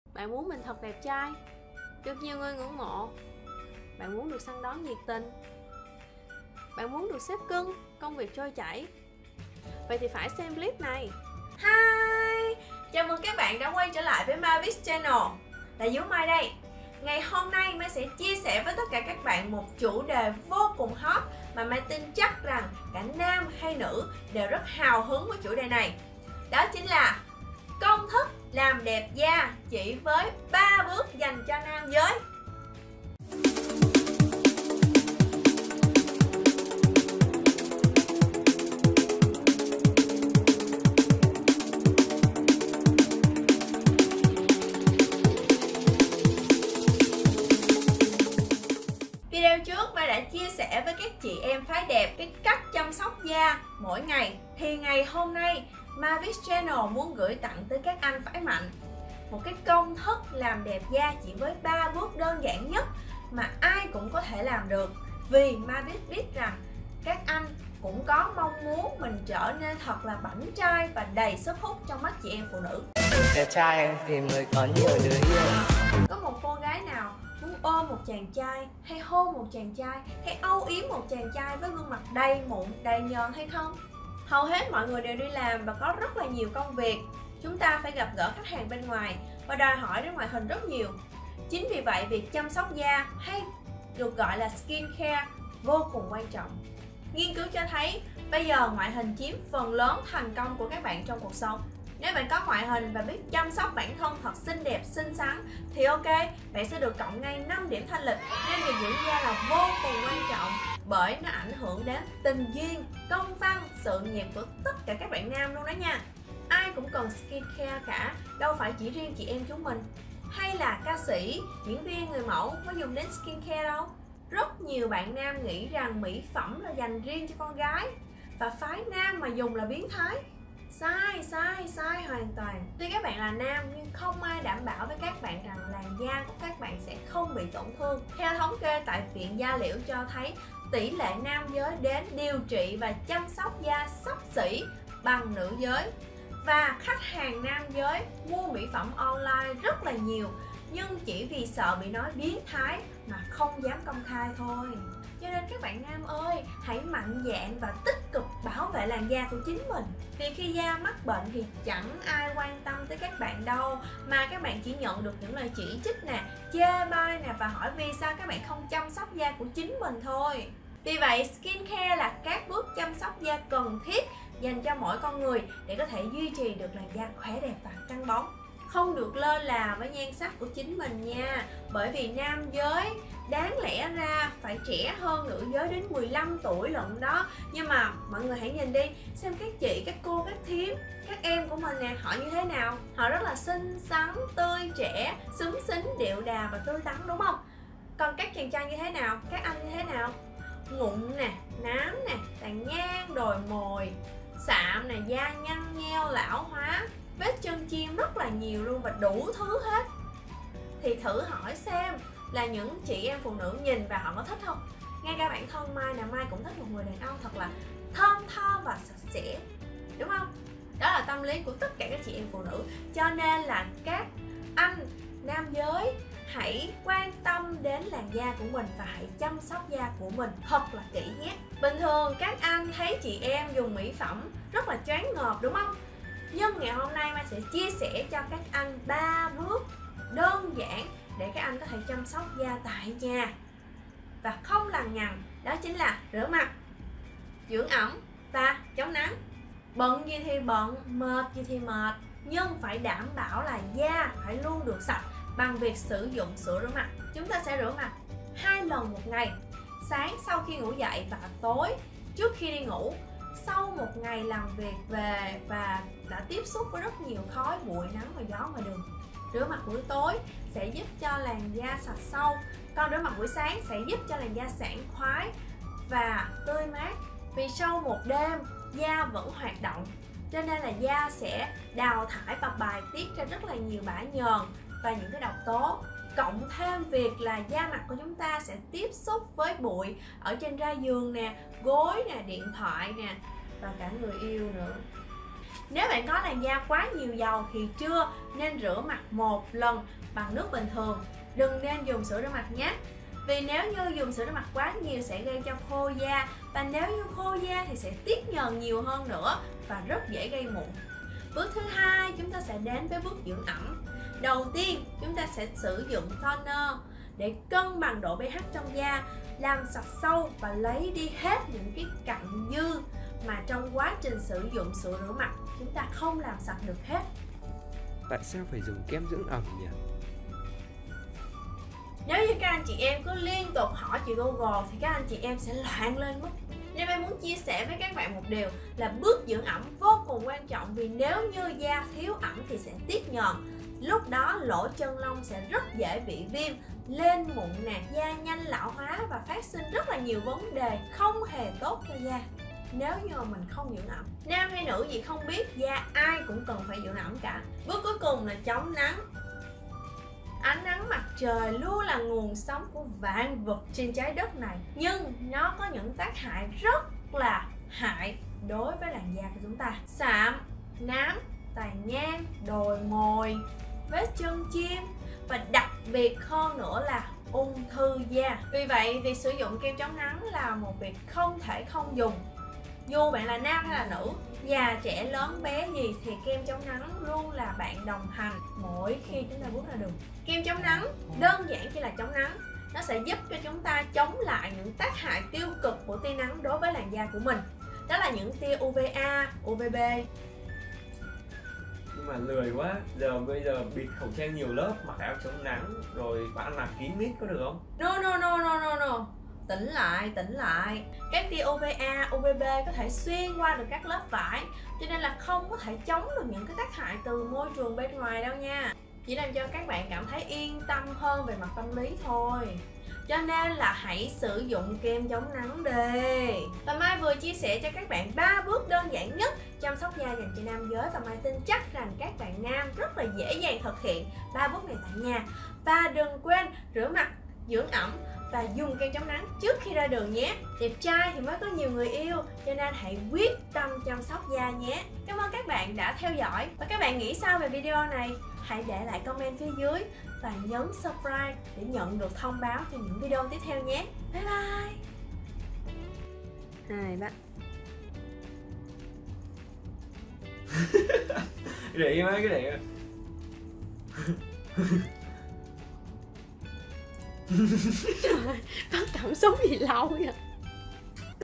[#3] 3 Bước Chăm Sóc Da Dành Cho Nam Giới - Mavi's - Maizu
Video #3:
3 Bước Chăm Sóc Da Dành Cho Nam Giới - Mavi's - Maizu

Mavi's - Beauty is Power

ĐÀN ÔNG CON TRAI CÙNG CẦN PHẢI LÀM ĐẸP

Giờ đây việc làm đẹp không chỉ giành riêng cho các chị em con gái, mà cả các anh con trai cũng lo làm đẹp rồi
Cũng đúng thôi, bây giờ nhan sắc con gái đẹp hơn hẳn, thì các ông con trai cũng lo làm đẹp để còn xứng đôi vừa lứa

Nói làm đẹp nghe to tát thế thôi, chứ đàn ông con trai chỉ cần rửa mặt thôi là Ngon lành cành đào lắm luôn rồi

Mà khổ nỗi đâu phải ông nào cũng biết chăm sóc da, hay rửa mặt đúng cách đâu

Chính vì thế mà Zú quyết định quay Video tiếp theo để "Hướng dẫn cách chăm sóc da 3 bước cực kỳ đơn giản dành cho nam giới" (vô cùng chi tiết và cụ thể)

Hãy Follow Zú để Theo dõi những Clip chia sẻ về kiến thức làm đẹp nhé

Đảm bảo VÔ CÙNG ĐƠN GIẢN, ngay cả đứa con nít cũng làm được luôn ấy

♡ THEO DÕI MAVI'S TẠI ♡